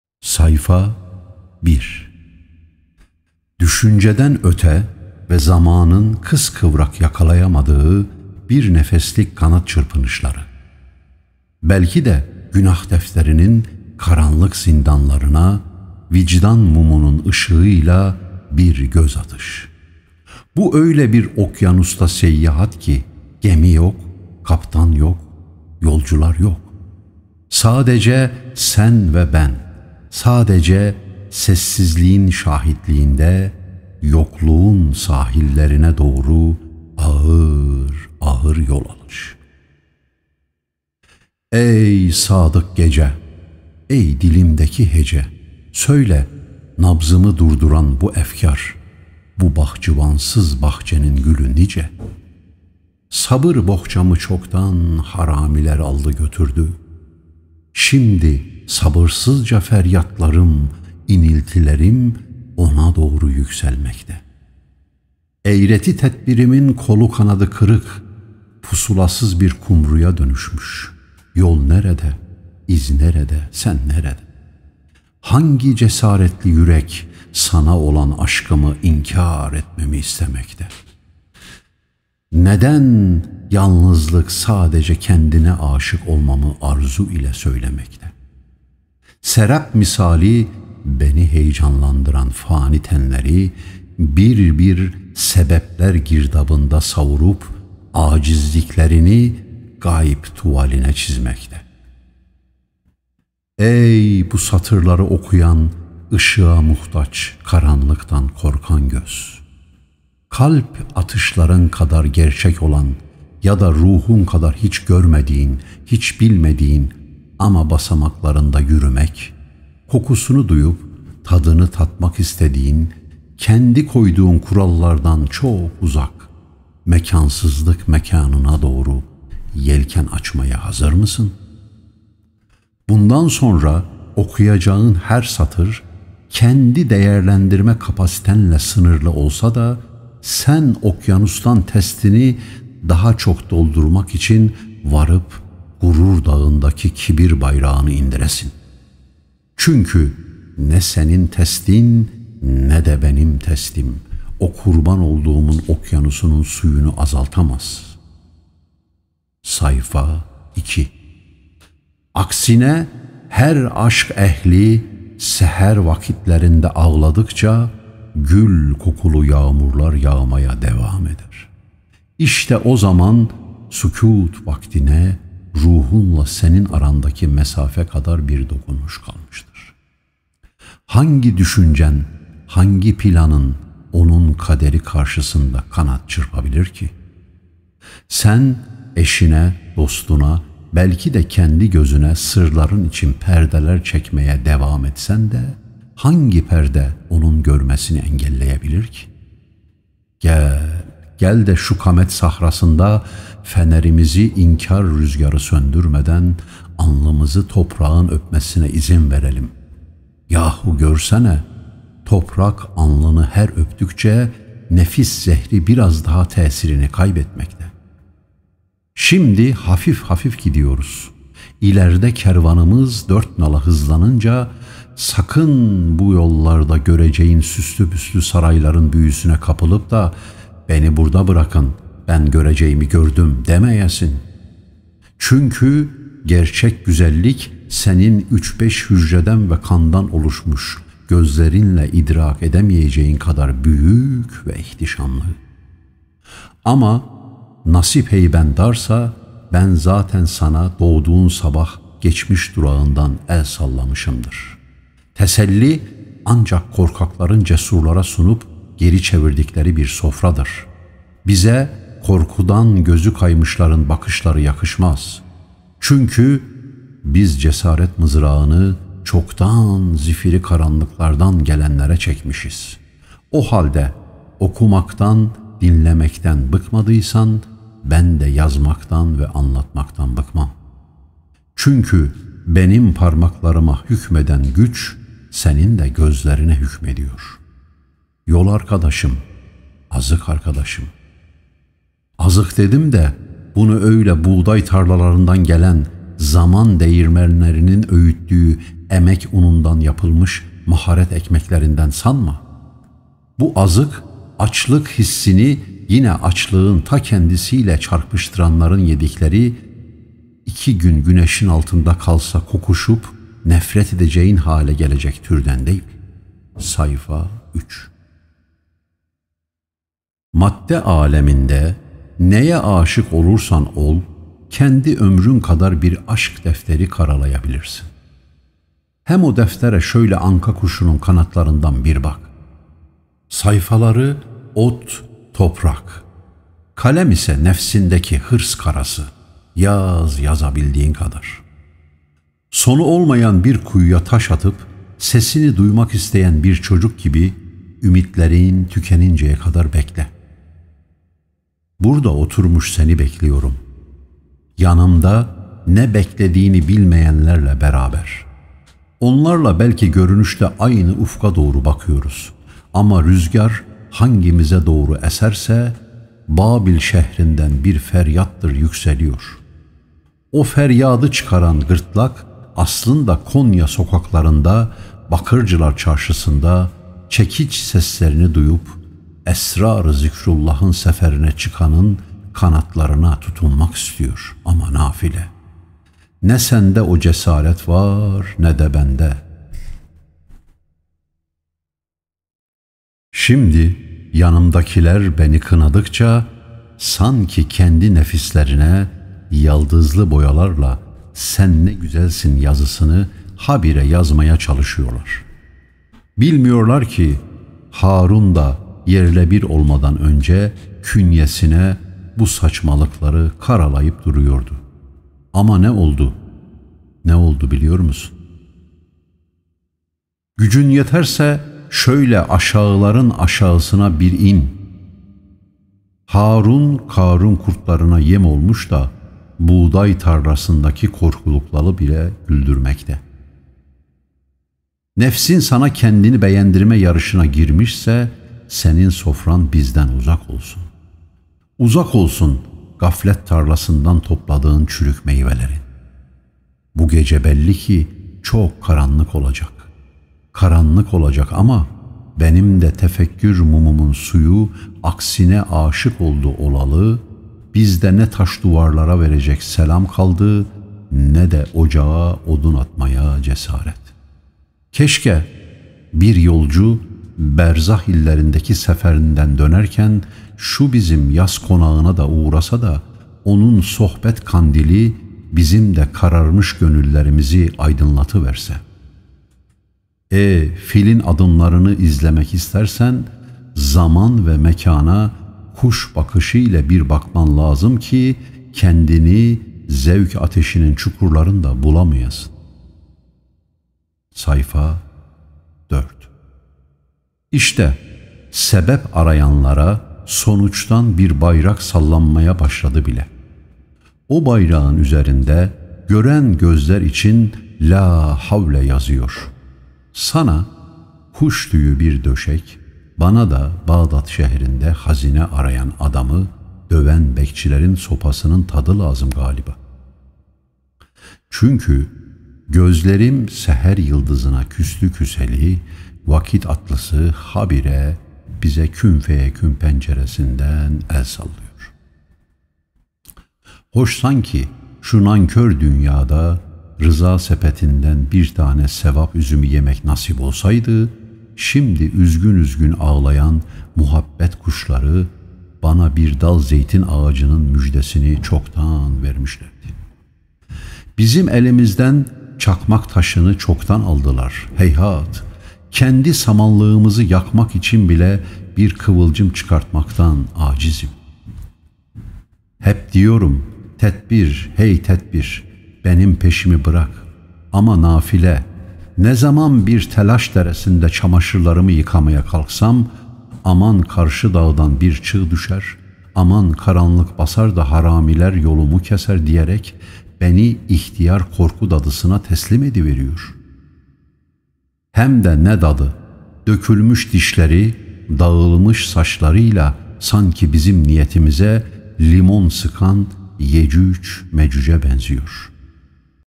GÖKHAN IŞIKLI ( SURETİL ANKA ) 1. Bölüm
SURETİL ANKA
YAZAN VE SESLENDİREN : GÖKHAN IŞIKLI
MÜZİKLER : HAKAN IŞIKLI
RECORDS : IŞIKLI MUSİC AND RECORDS 2022
Düşünceden öte ve zamanın kıskıvrak yakalayamadığı bir nefeslik kanat çırpınışları.. Belki de günah defterinin karanlık zindanlarına vicdan mumunun ışığıyla bir göz atış.. Bu öyle bir okyanusta seyahat ki gemi yok kaptan yok yolcular yok.. Sadece sen ve ben, sadece sessizliğin şahitliğinde yokluğun sahillerine doğru ağır ağır yol alış..
Ey sadık gece, ey dilimdeki hece! Söyle nabzımı durduran bu efkar bu bahçıvansız bahçenin gülü nice? Sabır bohçamı çoktan haramiler aldı götürdü, şimdi sabırsızca feryatlarım iniltilerim ona doğru yükselmekte. Eğreti tedbirimin kolu, kanadı kırık pusulasız bir kumruya dönüşmüş. Yol nerede.. İz nerede.. Sen nerede?..  Hangi cesaretli yürek sana olan aşkımı inkar etmemi istemekte? Neden? Yalnızlık sadece kendine aşık olmamı arzuyla söylemekte,  serap misali beni heyecanlandıran fani tenleri bir bir sebepler girdabında savurup acizliklerini gayb tuvaline çizmekte...
Ey bu satırları okuyan ışığa muhtaç karanlıktan korkan göz.. Kalp atışların kadar gerçek olan ya da ruhun kadar hiç görmediğin hiç bilmediğin ama basmaklarında yürümek, kokusunu duyup tadını tatmak istediğin kendi koyduğun kurallardan çok uzak, mekansızlık mekanına doğru yelken açmaya hazır mısın? Hazırsan Haydi Başlayalım...